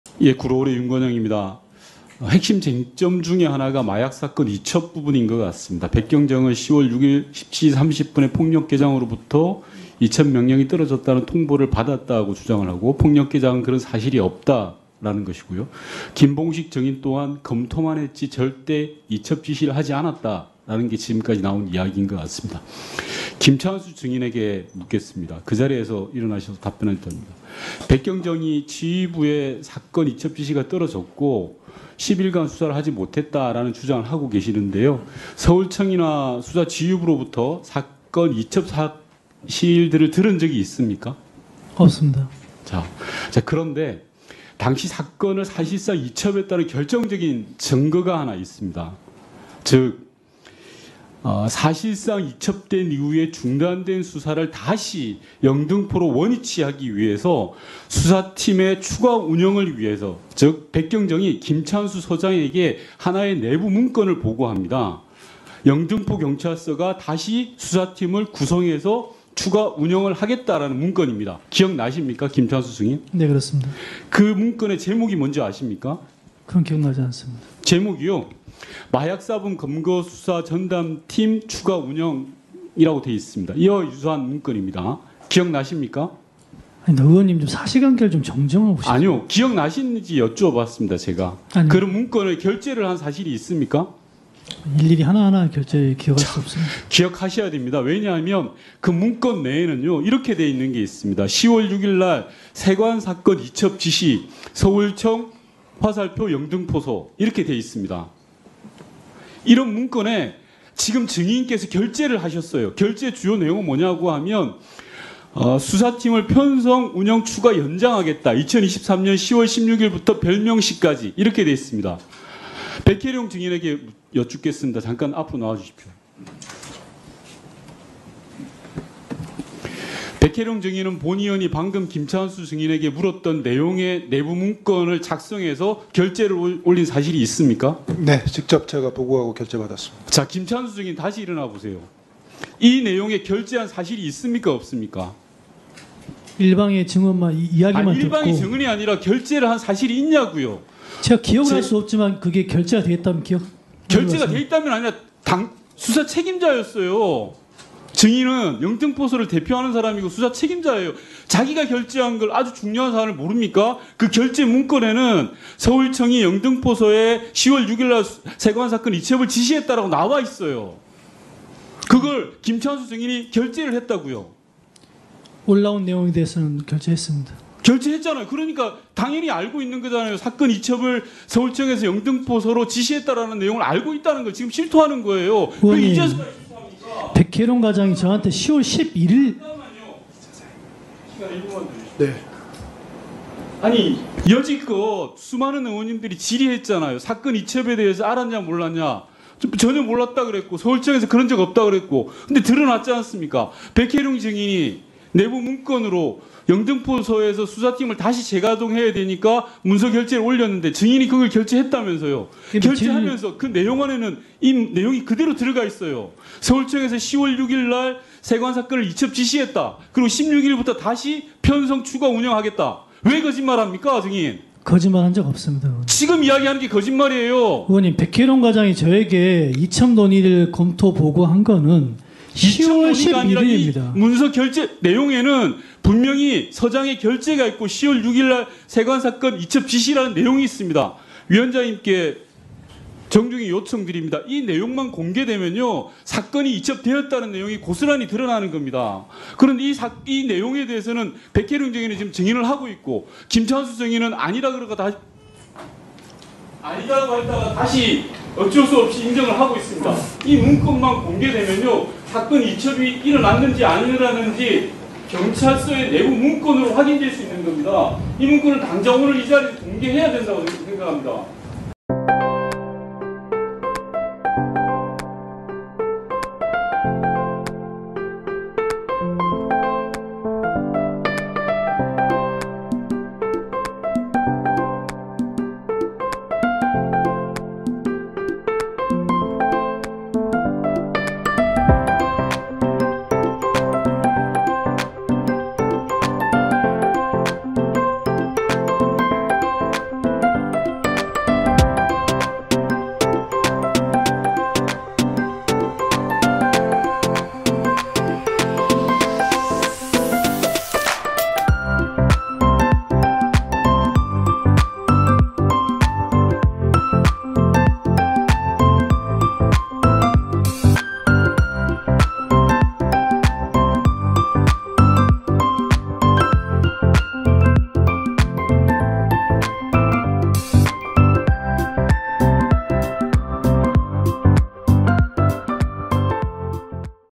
이 문건 공개되면 넌 끝났어!..윤건영 한 방에 영등포서장 좌절
2024년 8월 20일 국회에서
행정안전위원회 - 세관 마약 수사외압 의혹 청문회가 진행되었다.